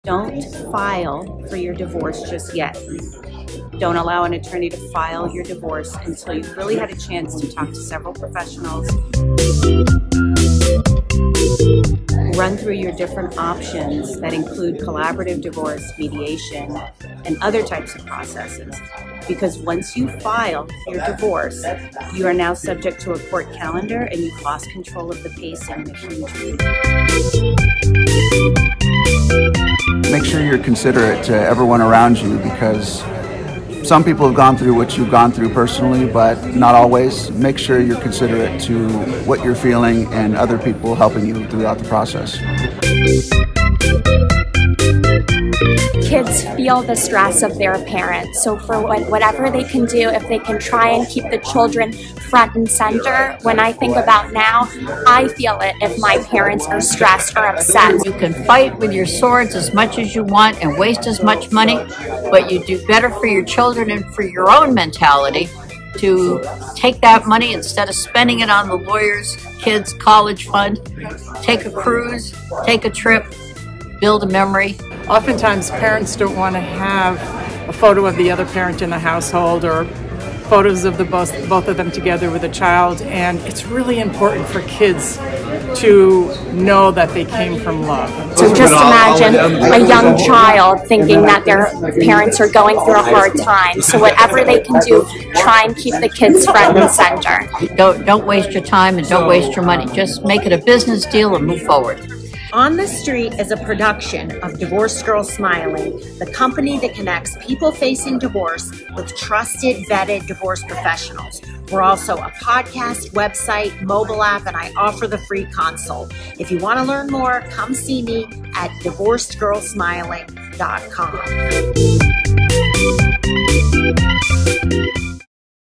On the Street Episode #11: Divorce Advice from Some of the Best Divorce Professionals!
I walked around the AFCC conference asking some wonderful divorce professionals to offer their divorce advice.